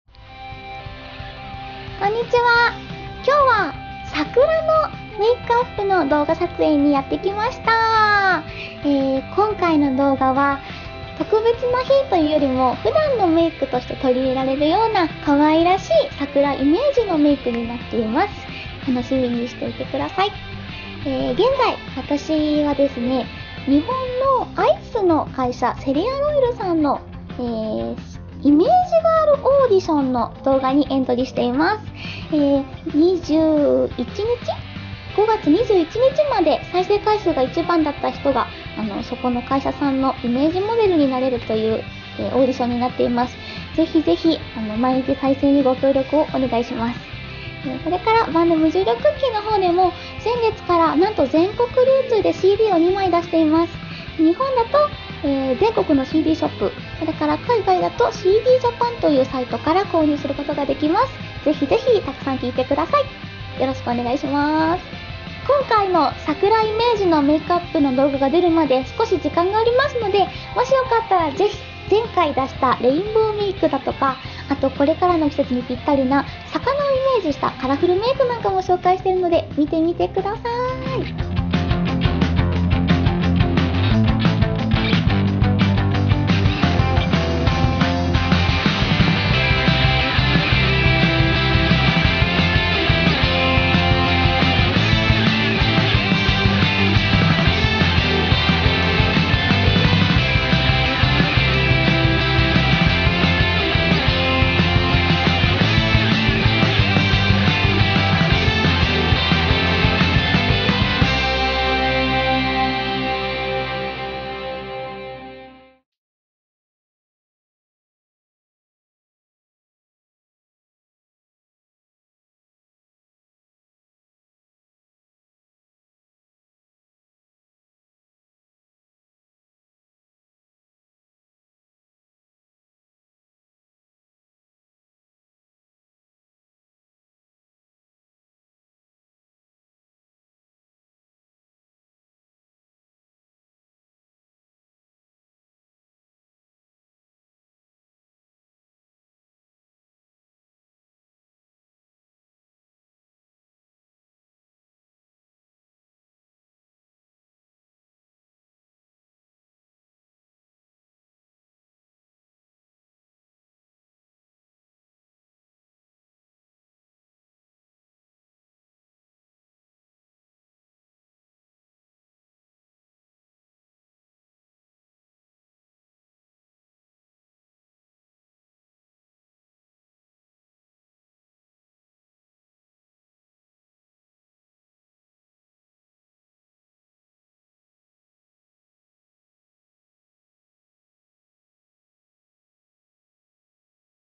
【Vlog】Cherry Blossoms Makeup preview
Today's Haruka Kurebayashi's SAKURA MAKEUP shooting preview

▼KAWAII♡PATEEN
Kawaii Makeup Tutorials｜日本の「カワイイ」を発信